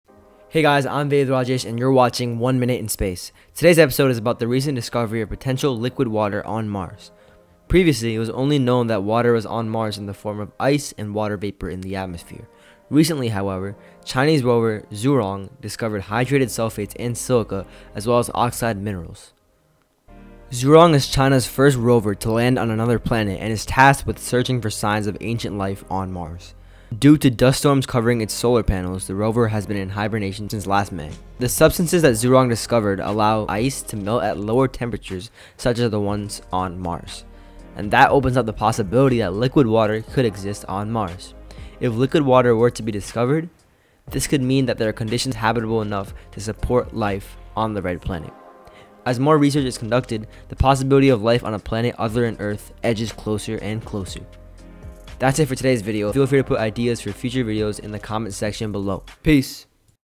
LIQUID WATER ON MARS?!
This episode of One Minute in Space is about the potential discovery of liquid water on Mars!